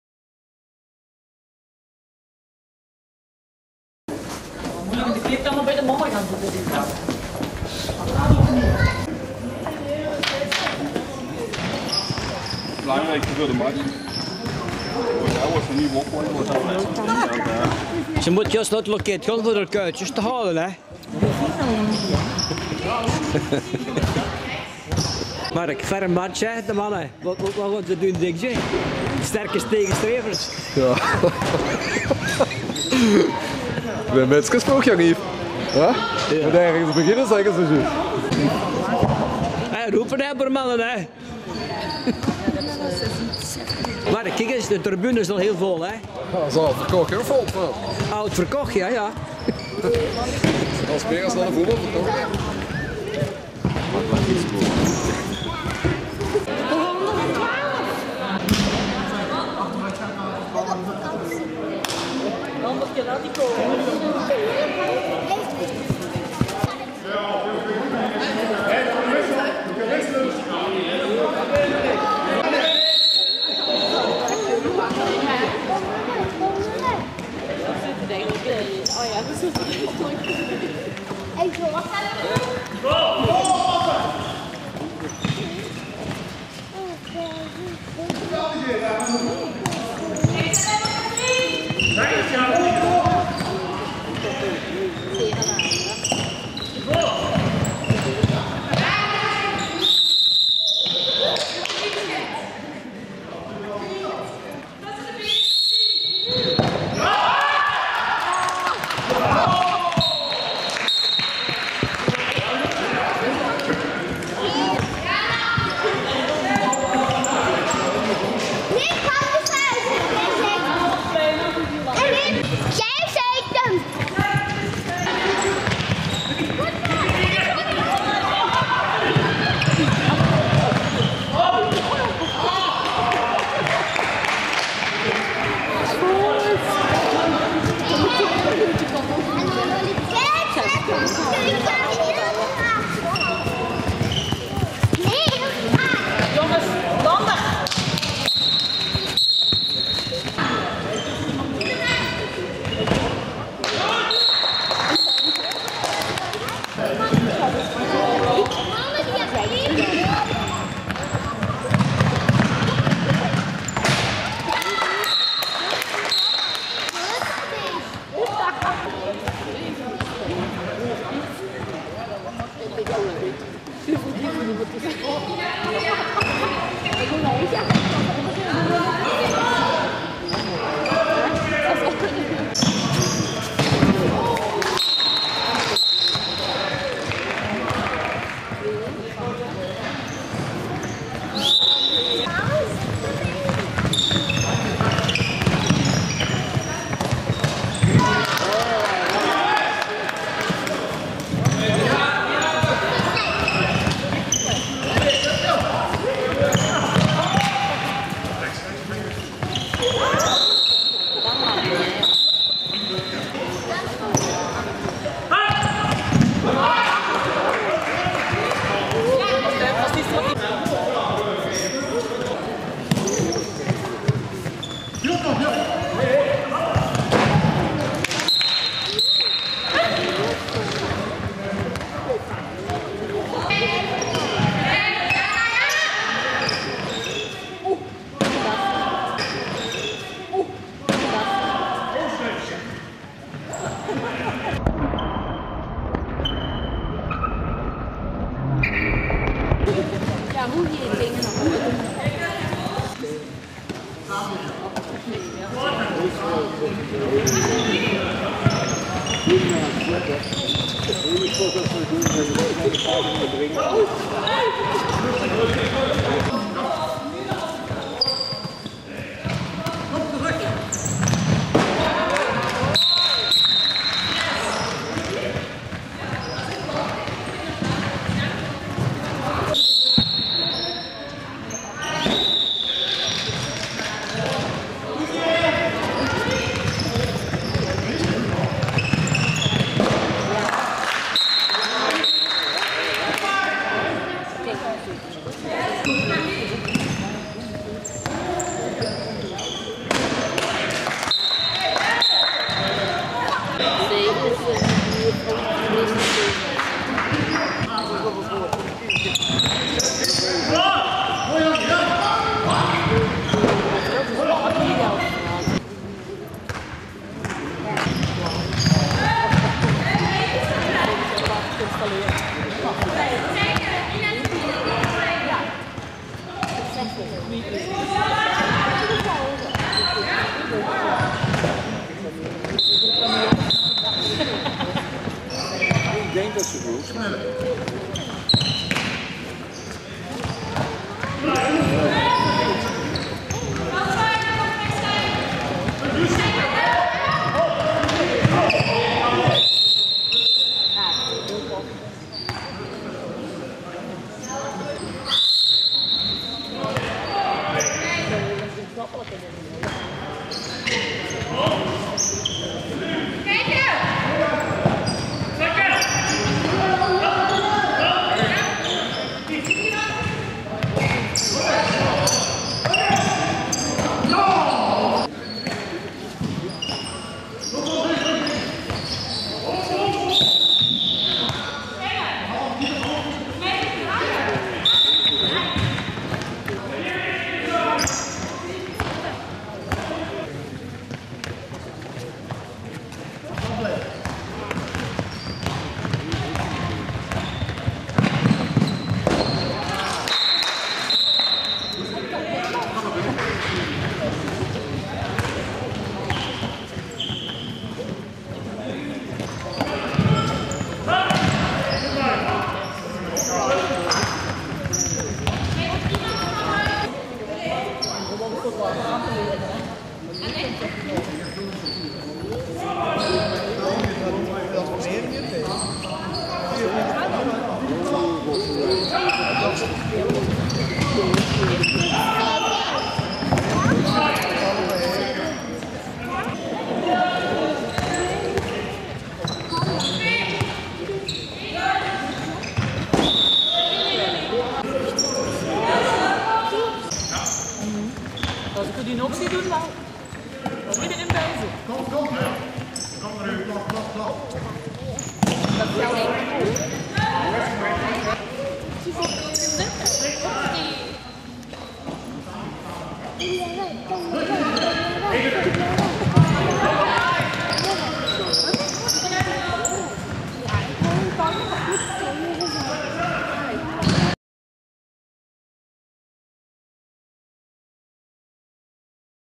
27 dec 2011 Hc de camionneurs Sportoase Tongeren - Handbal deel 1
Herenploeg (met "schoonzoon 1 en 2) tegen de Dames
Natuurlijk is dit niet de echte heren ploeg van Tongeren.